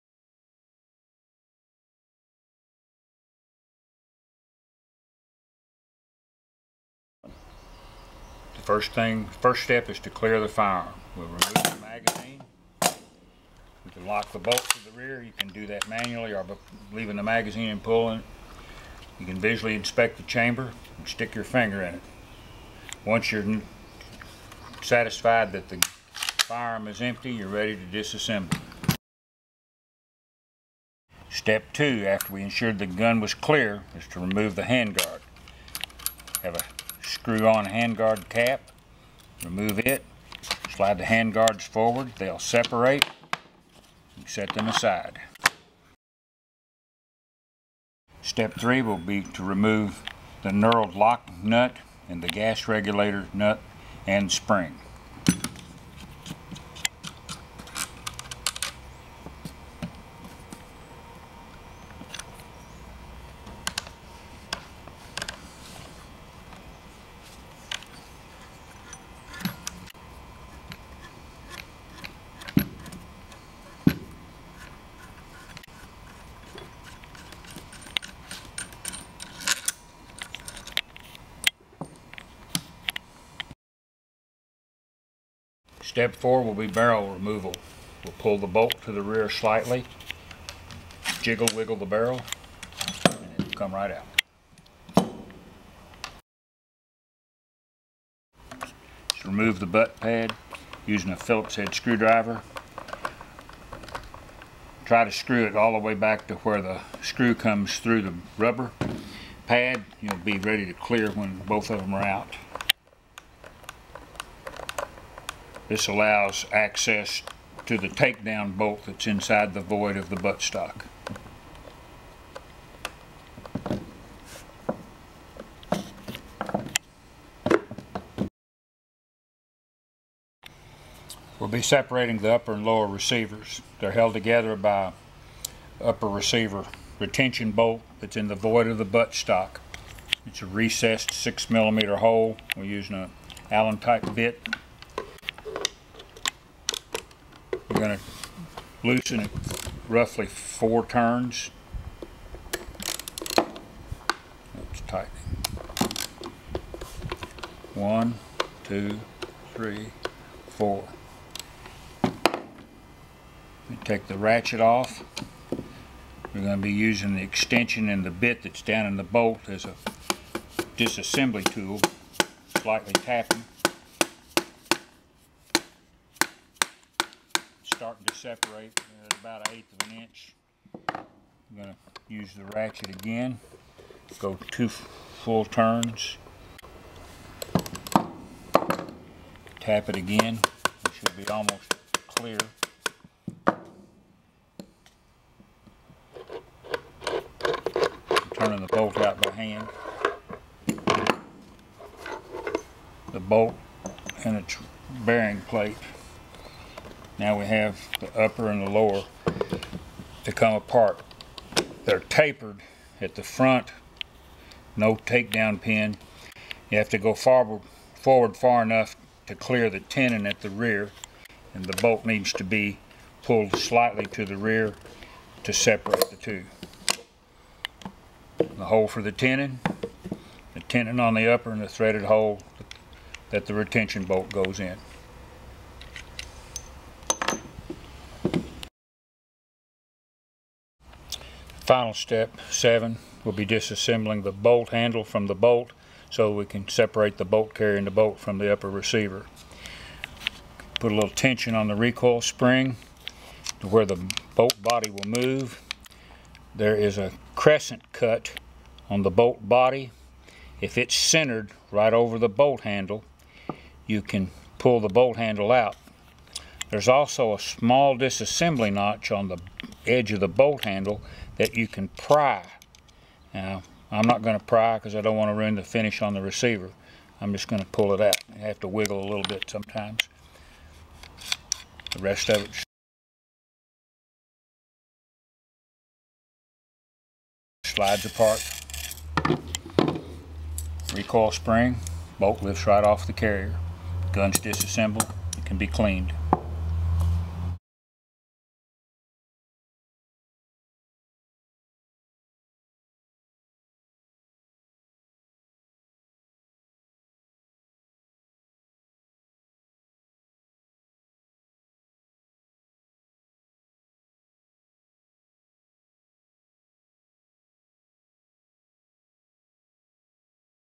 MKA 1919 DISASSEMBLY INSTRUCTIONS
Instructions on how to disassemble the MKA 1919 12 gauge shotgun.